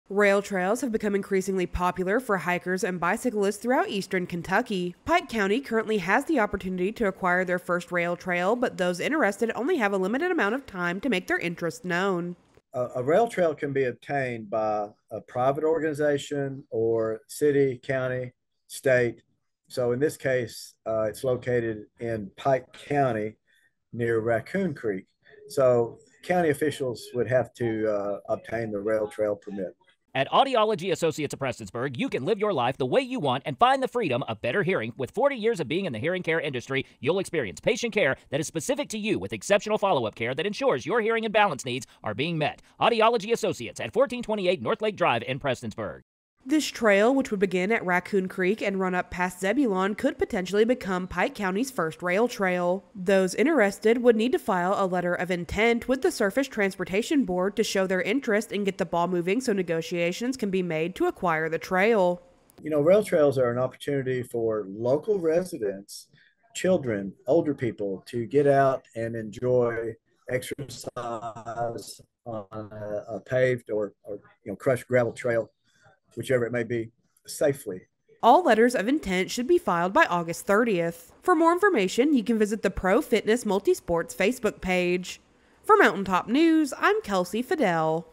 Pike County Could see its First Rail Trail
Pike County could see its first rail trail if a letter of intent is filed and approved.